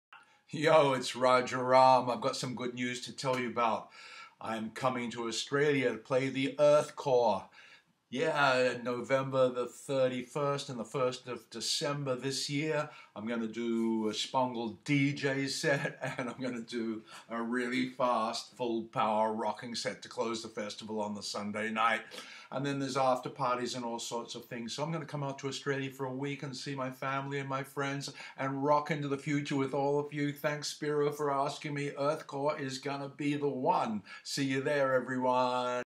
A message by Raja Ram from Shpongle to Australia
Introducing the headline act of earthcore 20 year anniversary. SHPONGLE DJ SET + Raja Ram Tip World 20 year anniversary closing set ! Tix/Info